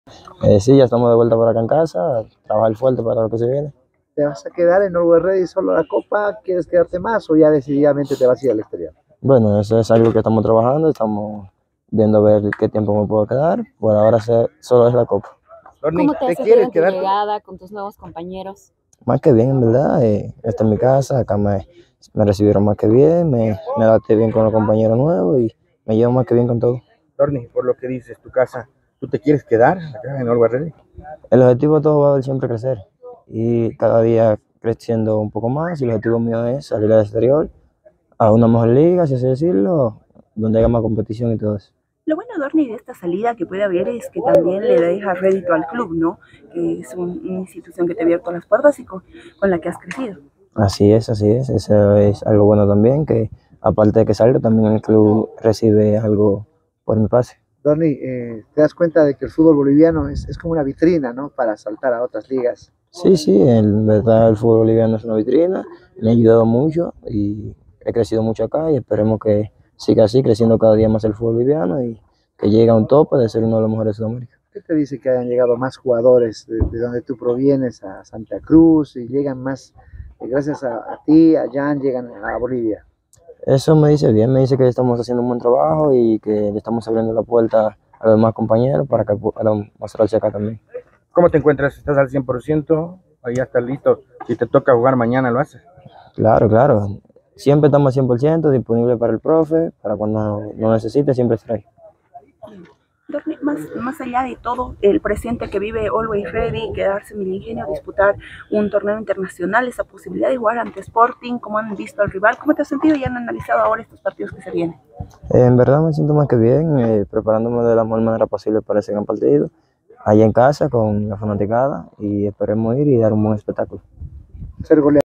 Dorny Romero delantero de Always Ready con miras al AMISTOSO ante The Strongest
Dorny Romero delantero de Always Ready | AMISTOSO ante The Strongest